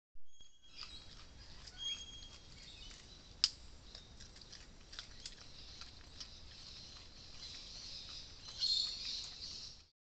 Gingernut eating carrot
The Friends of the Brush-tailed Rock-wallaby are funding the temporary supplement feeding of these endearing endangered creatures after the unprecedented drought and fires have devastated their rocky habitat. Help us help them by becoming a member, symbolically adopting a Kangaroo Valley Rock-wallaby or making a tax-deductible donation